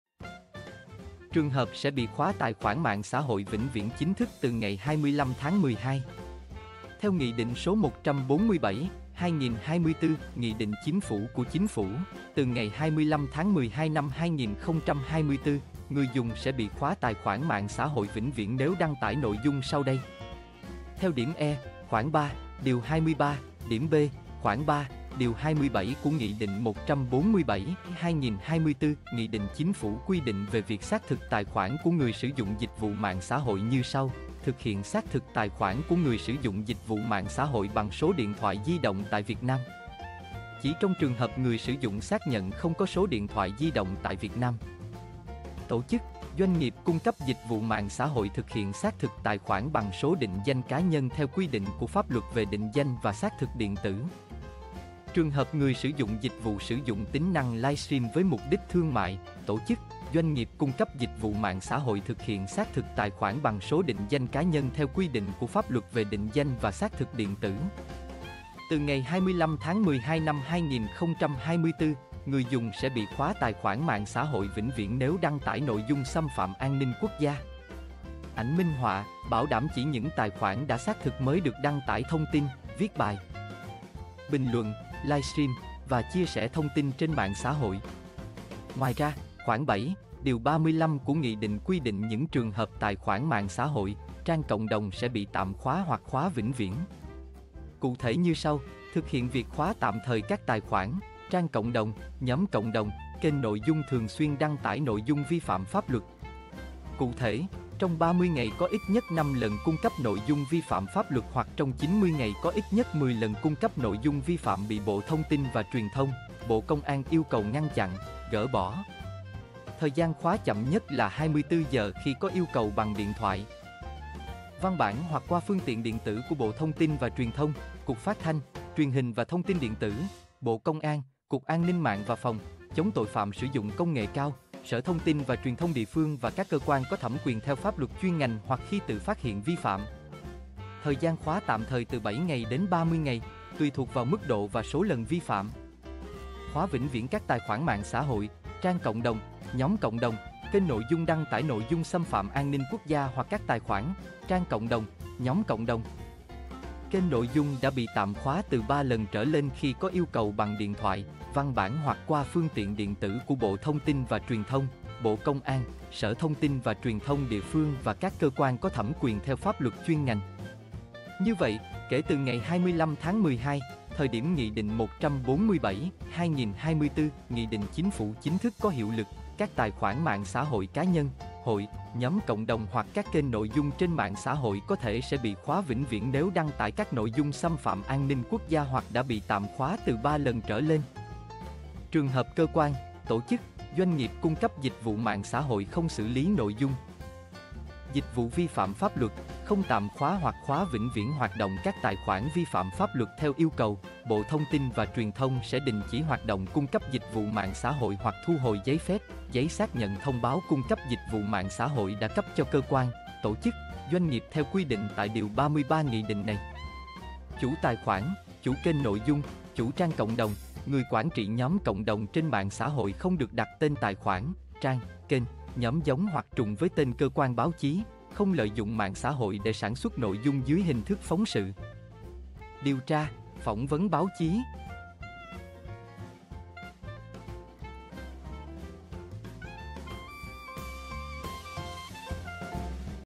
Trường hợp sẽ bị khóa tài khoản mạng xã hội vĩnh viễn chính thức từ 25/12
Theo Nghị định số 147/2024/NĐ-CP của Chính phủ, từ ngày 25/12/2024, người dùng sẽ bị khóa tài khoản mạng xã hội vĩnh viễn nếu đăng tải nội dung sau đây.